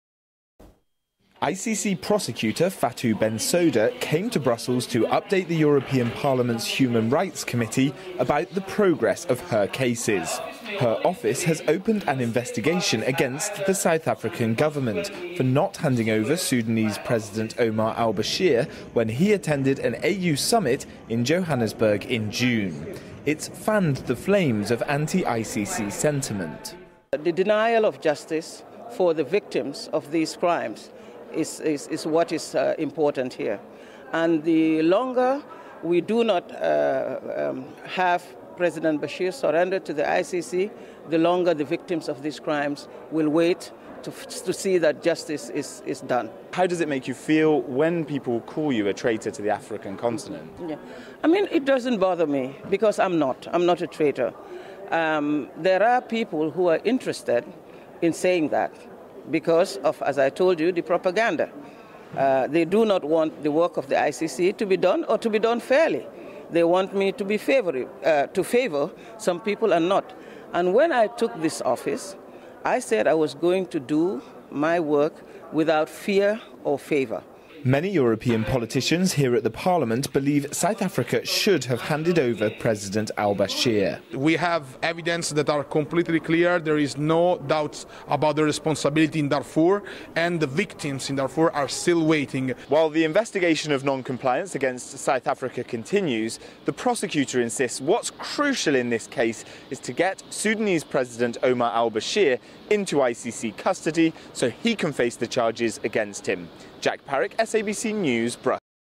Interviewed by the South African TV SABC News on Darfur
My comment for the South African TV SABC News on Darfur.